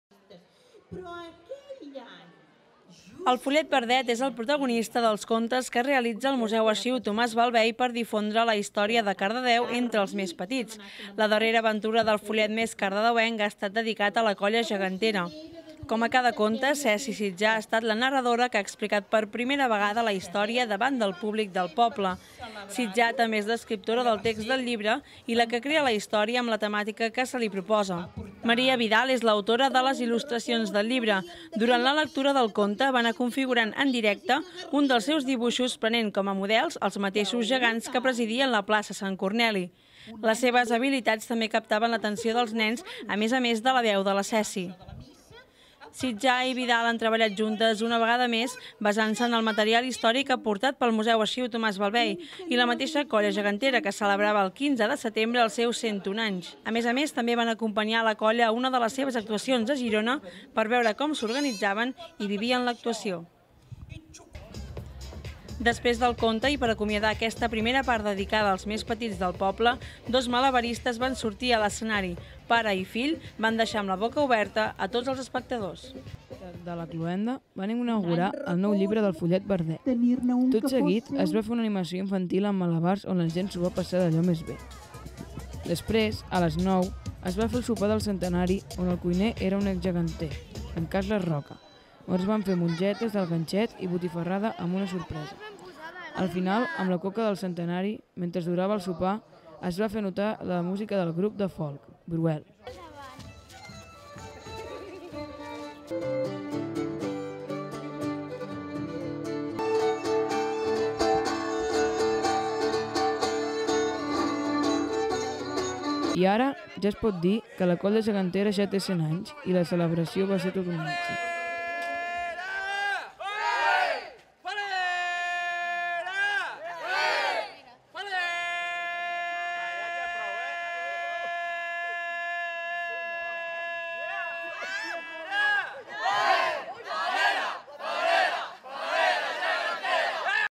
Els gegants clouen el centenari
Els Gegants de Cardedeu ja fa un any que estan de celebració. Precisament perquè fa un segle que existeixen a la nostra vil·la. Dissabte van fer la cloenda dels actes que han anat realitzant per celebrar-ho. El Follet Verdet va iniciar la tarda.
I continuem encara una mica més amb el centenari dels gegants, els contes i l'animació infantil van anar seguits d'un sopar i d'un concert.